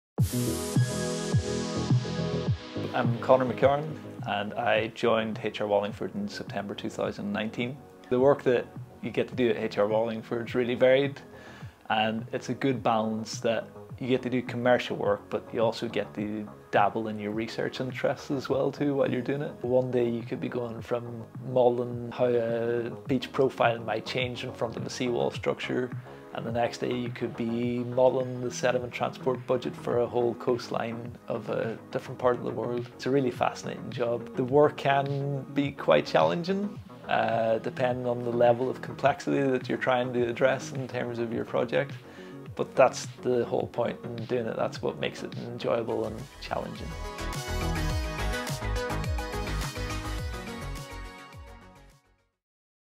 Meet Scientist Connor McCarron
Connor, a coastal and oceanographic scientist, works across a variety of research and commercial projects from wind farms to coastal protection. He enjoys the mix of hands-on testing and computer modelling, which allow him to really understand the data.